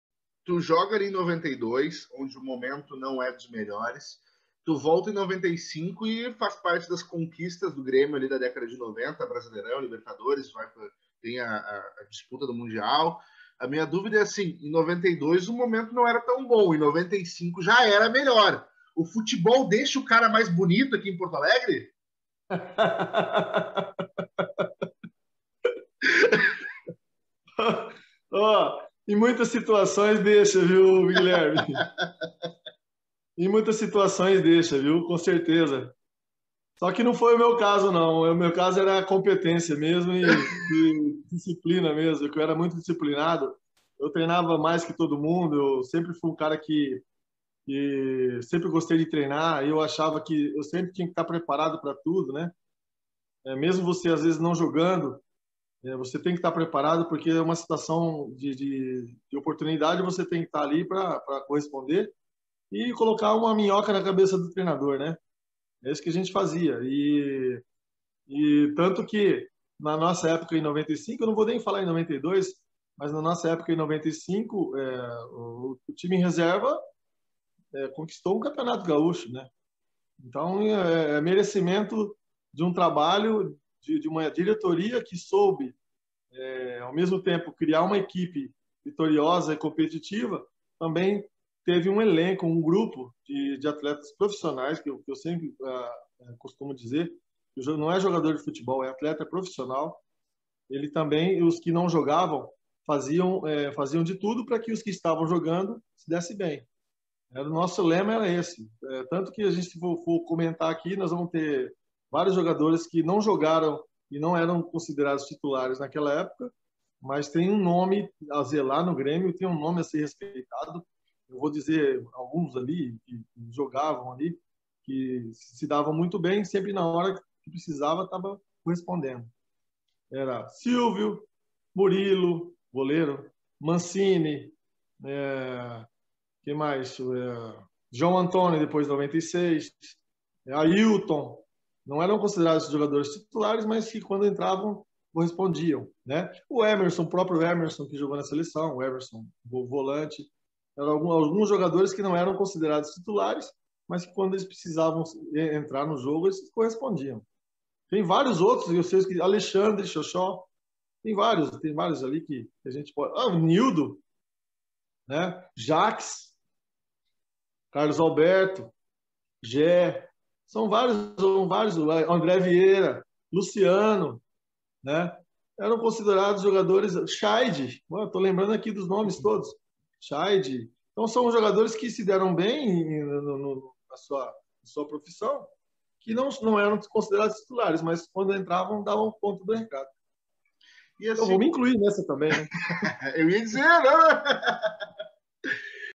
O ELENCO TRABALHADOR QUE DEU AS CONQUISTAS DOS ANOS 90
Wagner Fernandes viveu os tempos ruins e bons no Grêmio. Esteve no elenco da queda e também da glória. Aqui ele se recorda do perfil que o grupo gremista tinha.